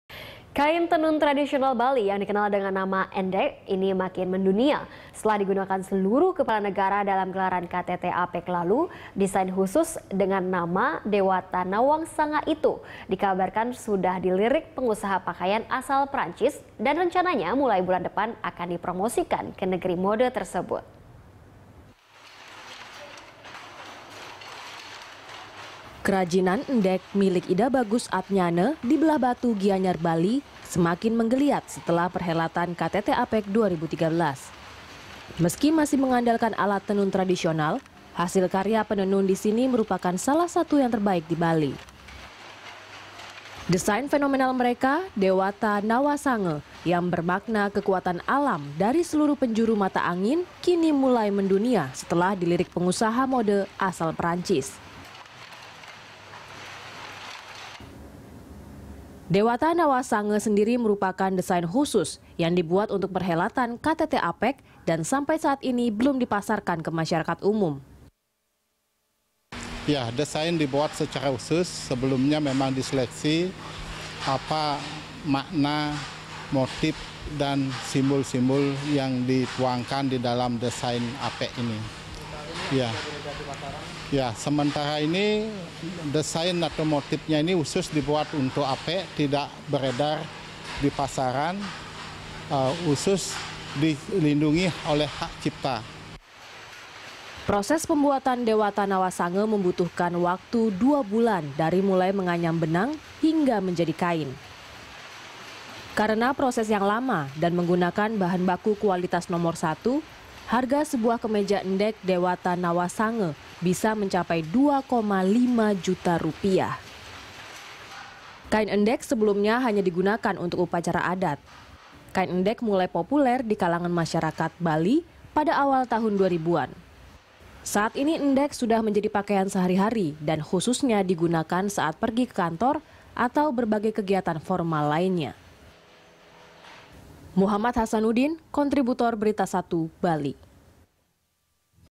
Pasca KTT APEC, Kain Endek Dikenal Dunia
Kain tenun tradisional Bali yang dikenal dengan nama Endek, kini makin mendunia setelah digunakan seluruh kepala negara dalam gelaran KTT APEC 2013 yang baru saja usai. Desain khusus dengan nama Dewata Nawasanga itu, dikabarkan sudah dilirik pengusaha pakaian asal Prancis. Rencananya, mulai bulan depan akan dipromosikan ke negeri mode tersebut.

Facebook.com/BeritaSatuTV
Youtube.com/BeritaSatu